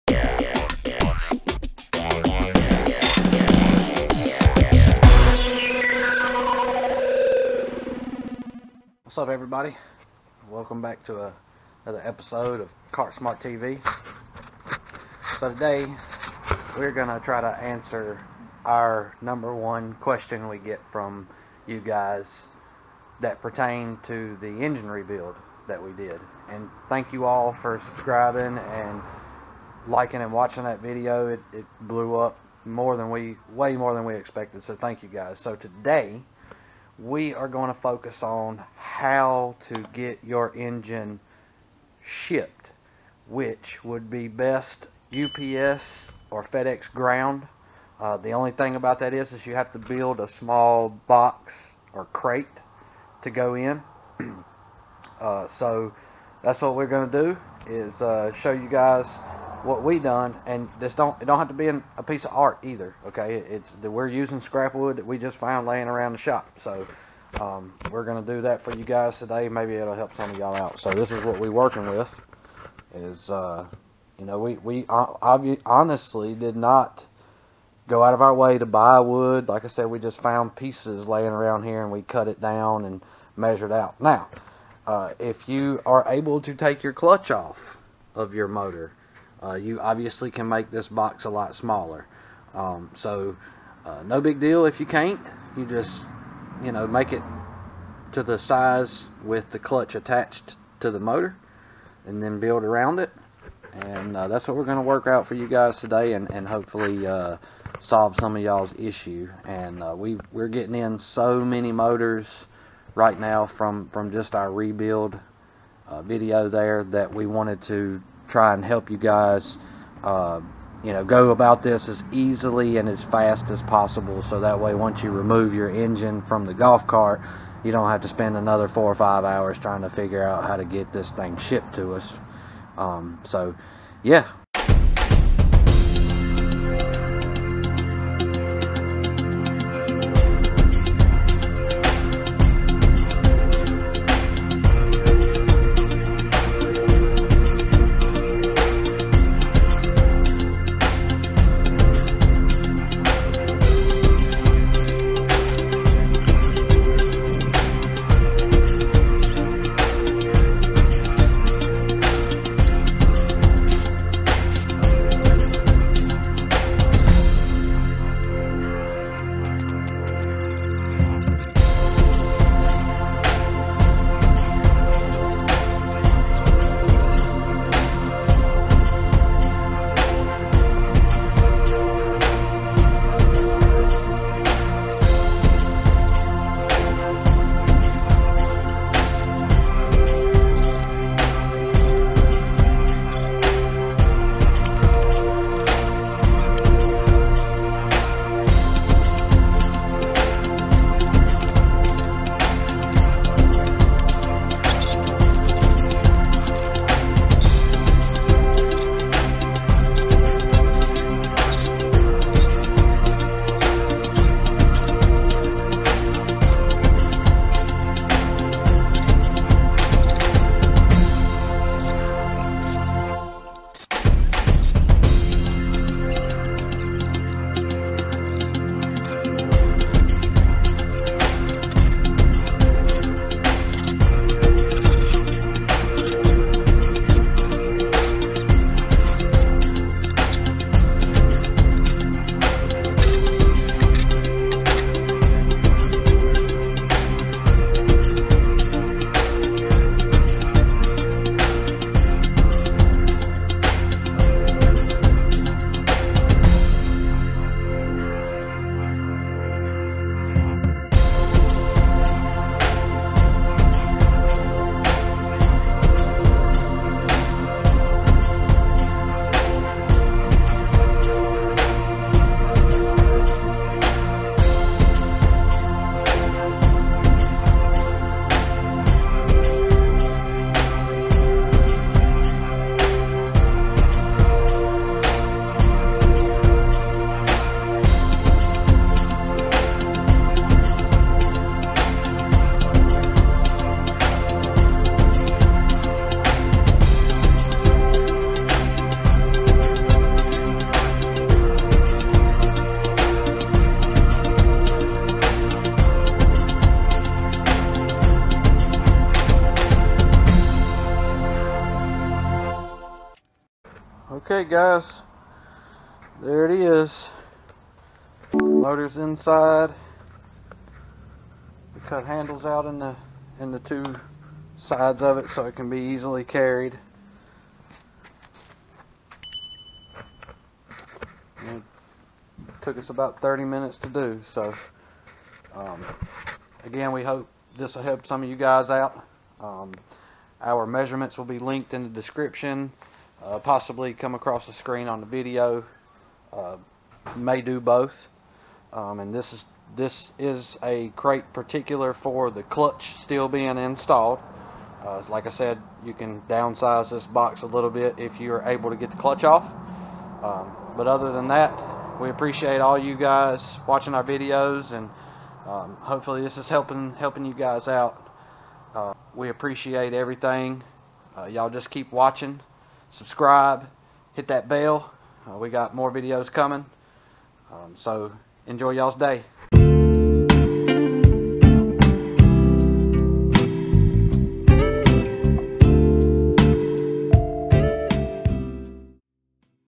How to Build an Engine Shipping Crate for EZGO
We show you How to Build an Engine Shipping Crate for EZGO engine. These can be shipped UPS or FedEx Ground. Since our engine rebuild video we have been getting a lot of engines sent to us to rebuild but, this has been a big question with phone calls. We hope this helps. Martinsville, Va. 24112